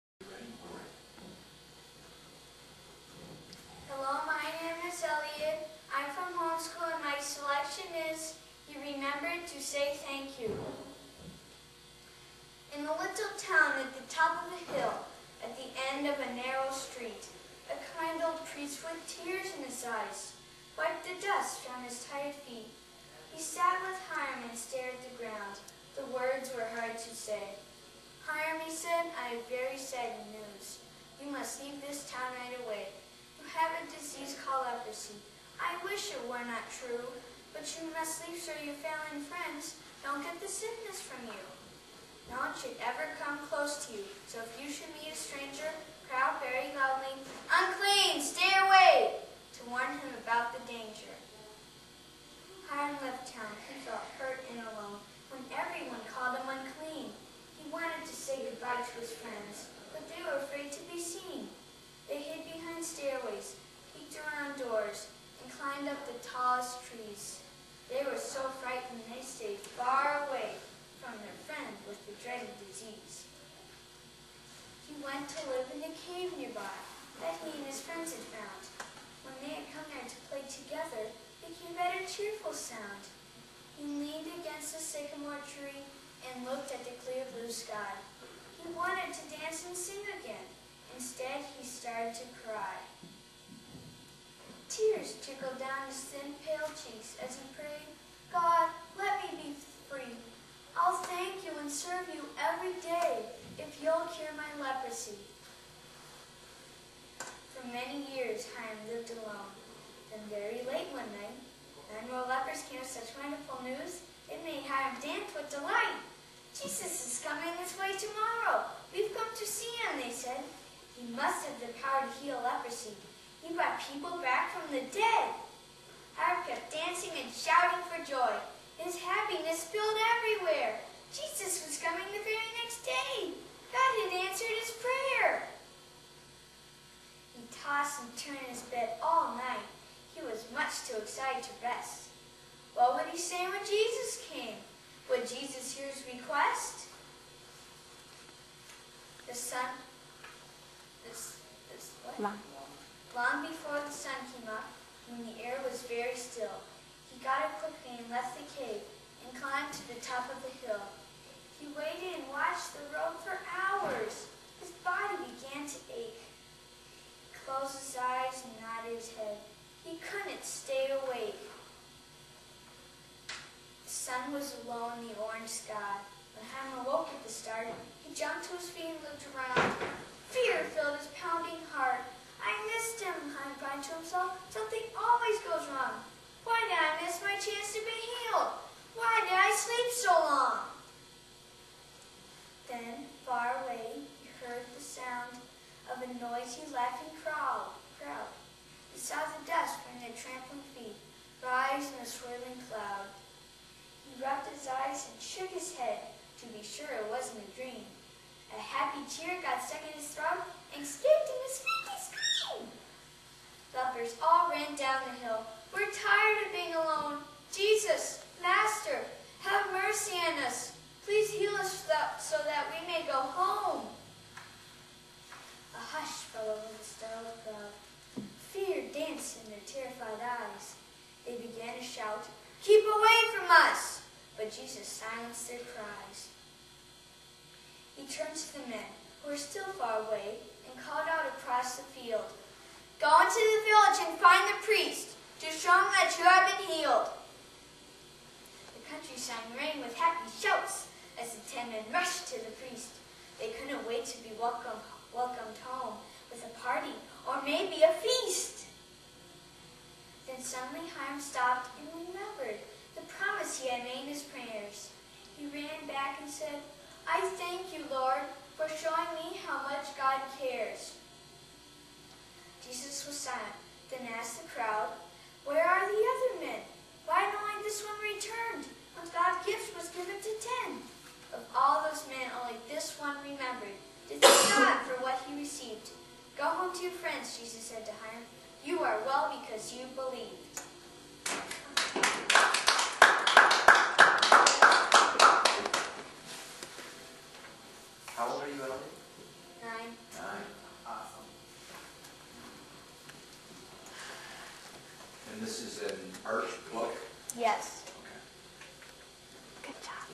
Elliot Jordan Poetry Recitation
Elliot won first place with this presentation of an Arch Book.  Enjoy!